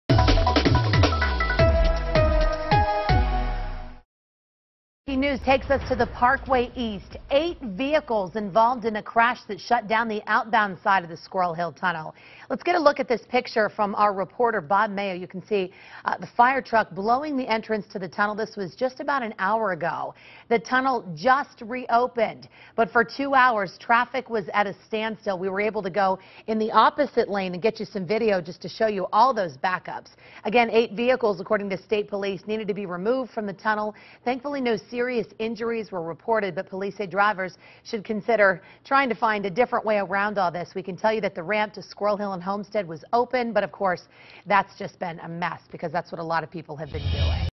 Squirrel Hill Tunnel reopens after crash
Several vehicles were involved in a crash that shut down the outbound Squirrel Hill Tunnel Tuesday morning.